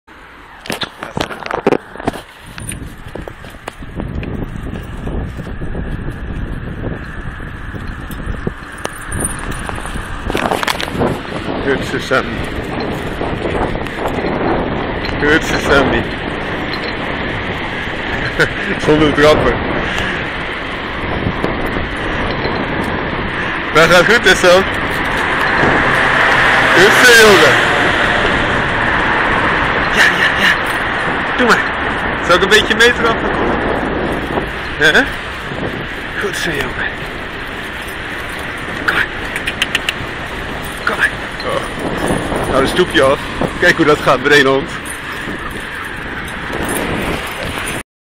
By Spartan Law: Irish Staffordshire Bull Terrier BSL's Samson age 7 on the bike through tunnel
By Spartan Law: Irish Staffordshire Bull Terrier BSL's Samson age 7 on the bike in the Netherlands